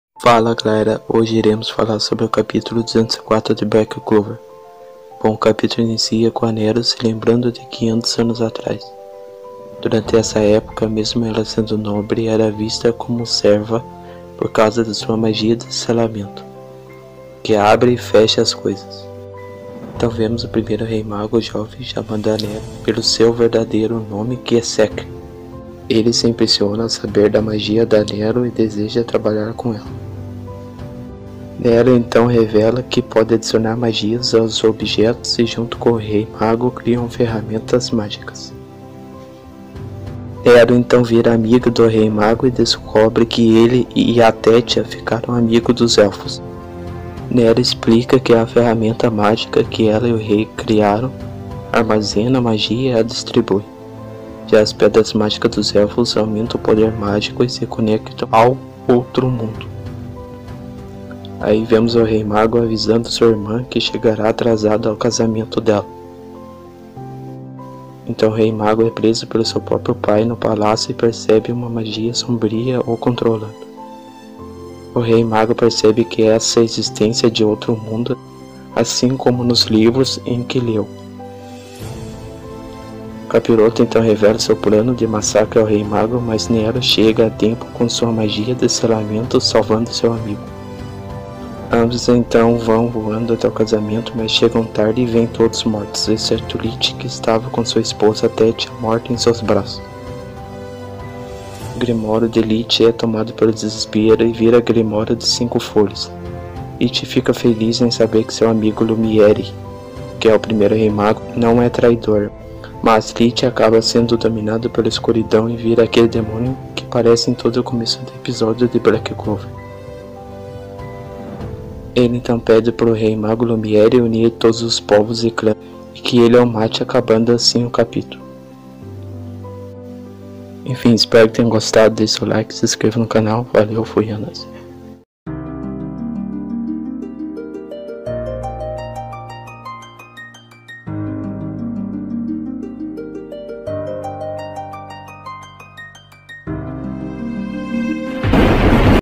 BLACK CLOVER CAPITULO 204- PRIMEIRO REI MAGO LUMIERE E EXPLICAÇÕES DE COMO OS ELFOS FORAM MORTOS
Nesse vídeo comento sobre o capítulo 204 do mangá de Black Clover